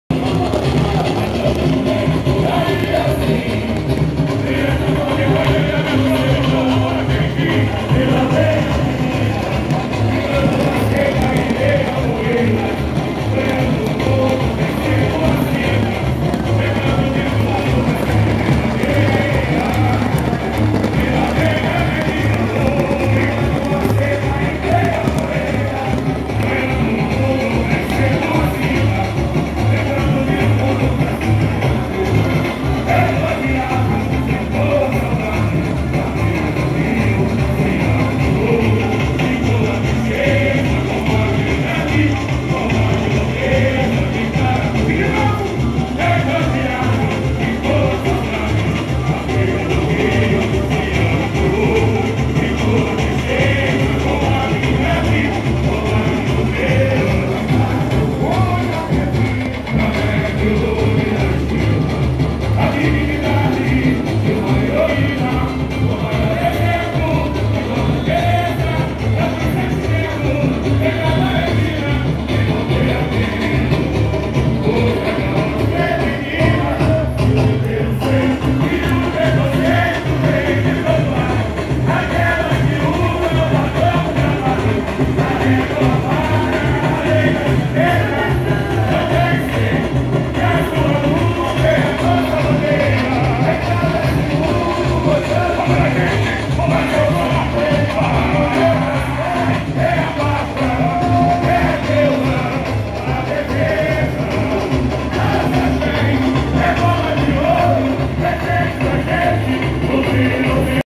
INOCENTES 2020 - ALEGORIA 01 – ABRE-ALAS - ALAGOAS, SUA MAIOR PAIXÃO
MUSA
LUCIENE SANTINHA
"FLOR DO NORDESTE"

ALEGORIA 01 – ABRE-ALAS - ALAGOAS, SUA MAIOR PAIXÃO

Sua fé na religião católica, a feira e a partida do ônibus para o Rio de Janeiro.